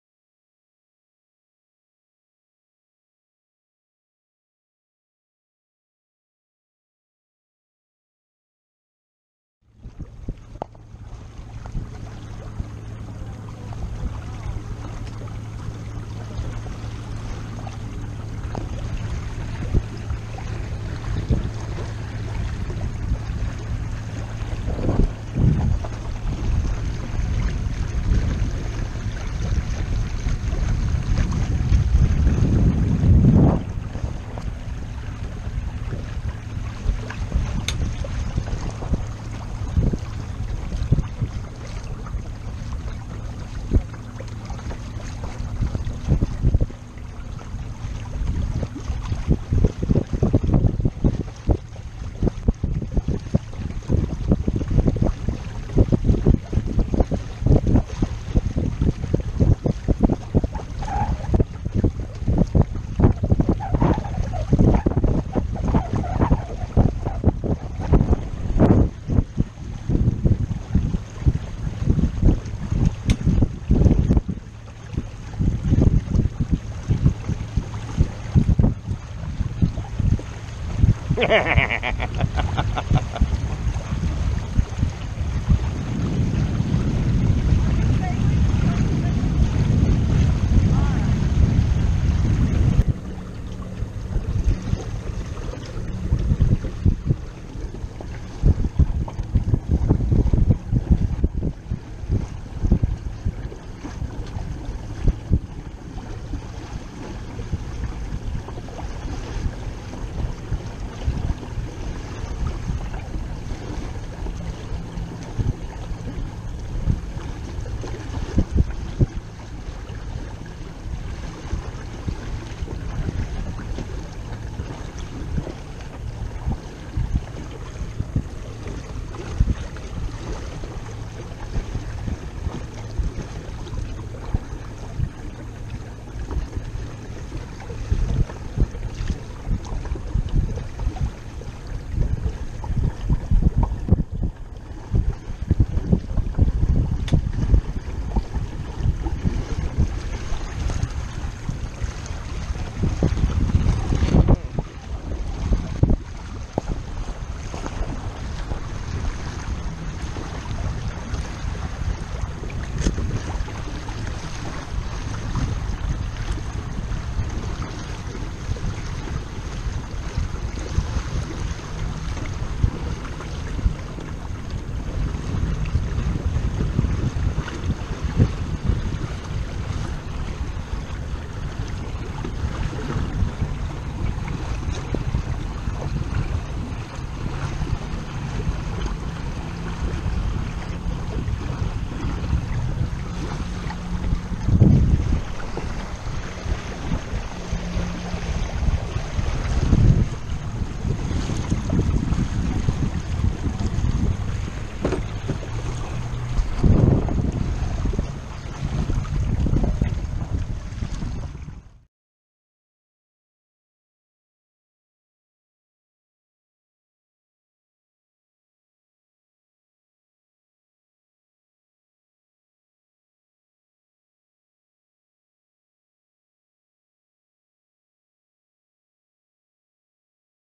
WindRider 16 Jib Speed Test
Coming back from a camping trip, I decided to do a speed test check while using the new jib. The jib is actually a genoa from a West Wight Potter 15. I've tried the boat using several other jibs since, including a WindRider 17 jib, which is larger than the Potter 15 genoa shown in this video and works even better in low winds.